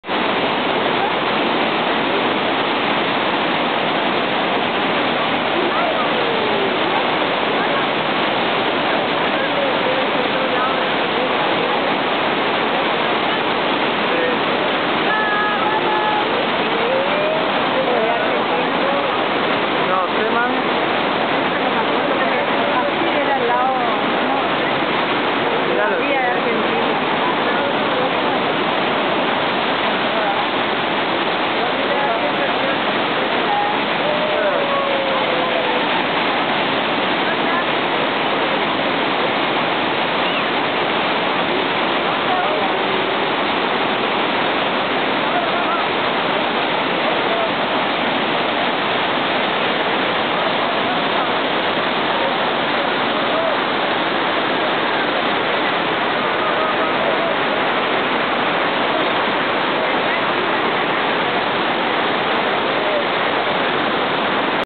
FASTANTICA CAIDA DE LA GARGANTA DEL DIABLO
FANTASTICA VISTA DE LA GARGANTA DEL DIABLO DE LAS CATARATAS DEL IGUAZU